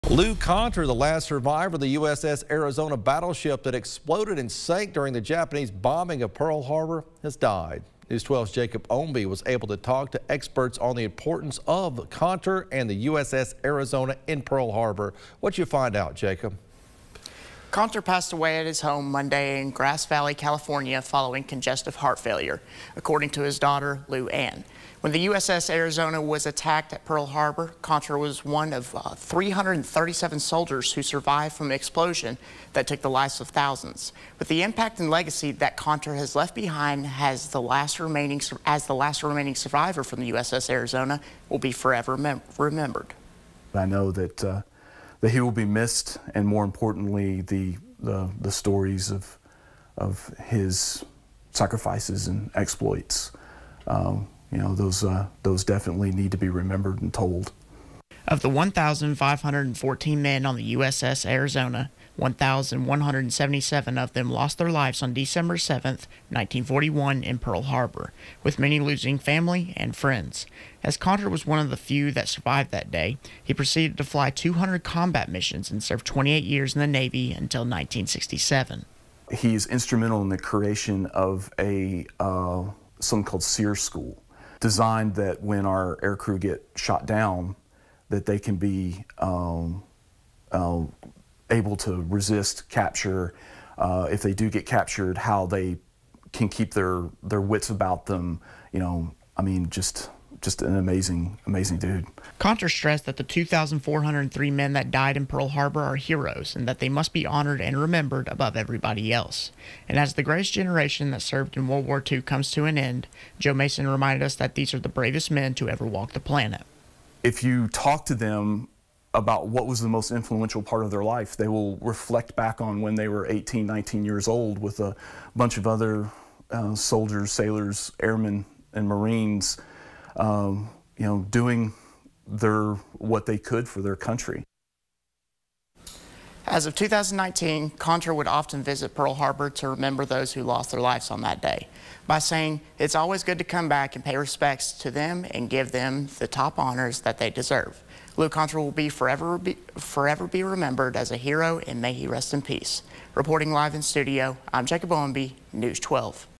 Last survivor of USS Arizona battleship in Pearl Harbor dies
In the wake of the passing of Lou Conter, News 12 takes a moment to highlight the importance of the USS Arizona battleship in Pearl Harbor.